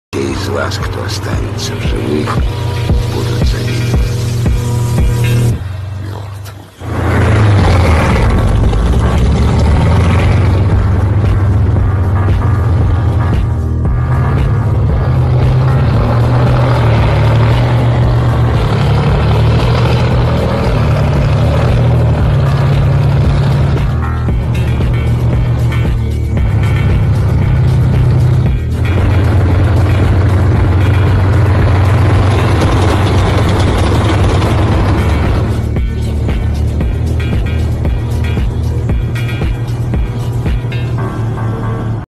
The T-54 and T-55 tanks
The T-54 and T-55 tanks are a series of Soviet main battle tanks introduced in the years following the Second World War. The first T-54 prototype was completed at Nizhny Tagil by the end of 1945.